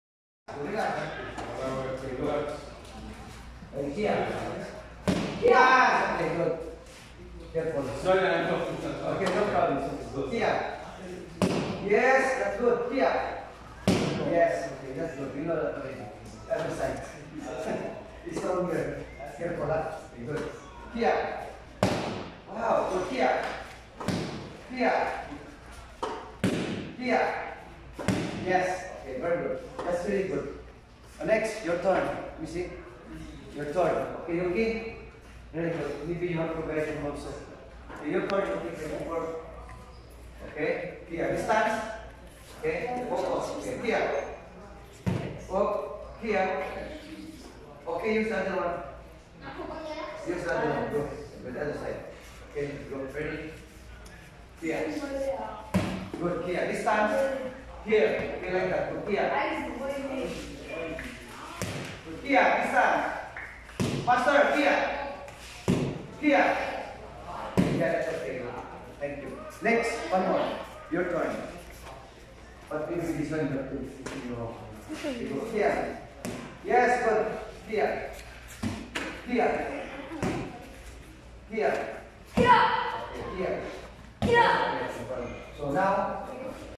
Train Mawashi geri biggeners left and right legs.
Train kicks using hips twisting feet twisting  both side left and  right legs.